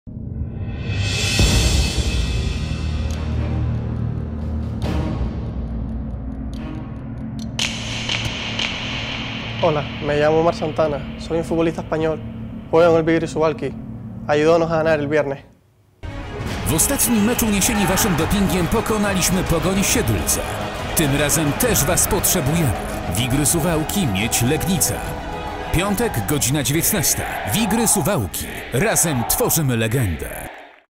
Zaproszenie | Wigry Suwałki - Miedź Legnica (29.04.2016)
29 kwietnia o 19.00 Wigry podejmą na suwalskim Zarzeczu plasującą się o jedną pozycję wyżej Miedź Legnica.